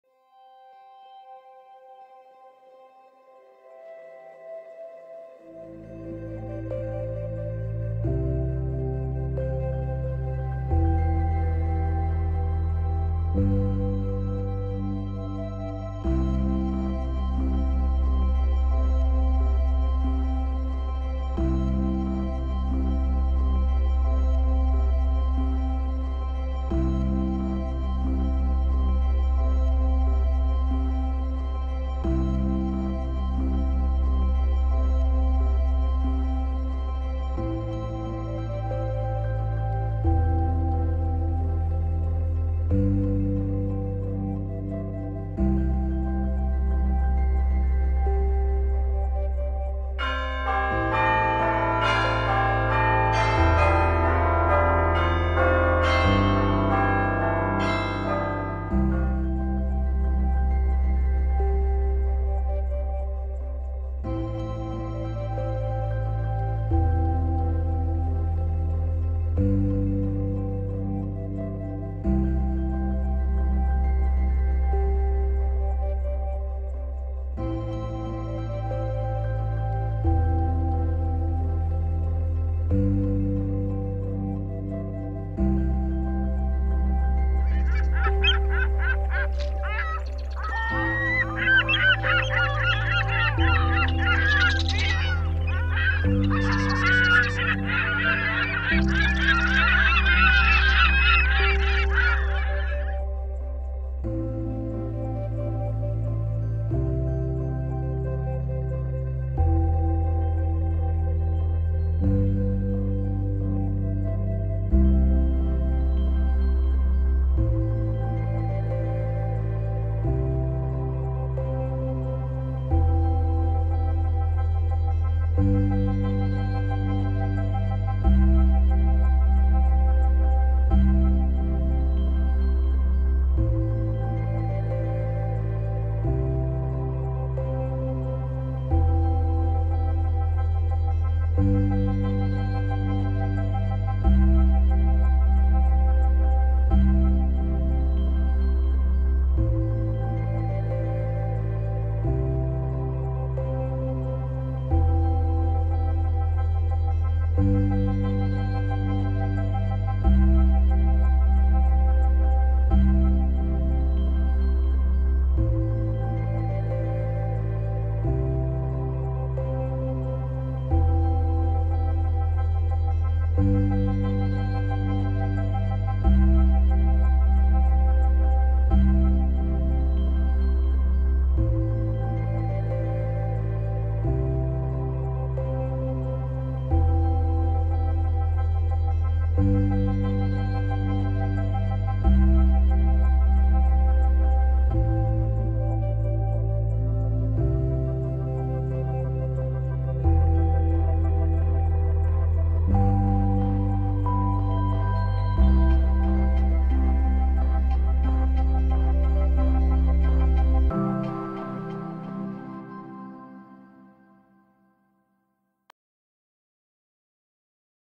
Laguna veneziana presepe nell'acqua 2022 Francesco Orazio. Visite guidate private di Venezia e isole
Questo anno l'artista Francesco Orazio ha deciso di allestire il presepe sull'acqua a Cavallino Treporti. Gli anni precedenti lo si poteva ammirare dall'isola di Burano.
Contattateci per scoprire i gioielli di Venezia e delle isole della laguna.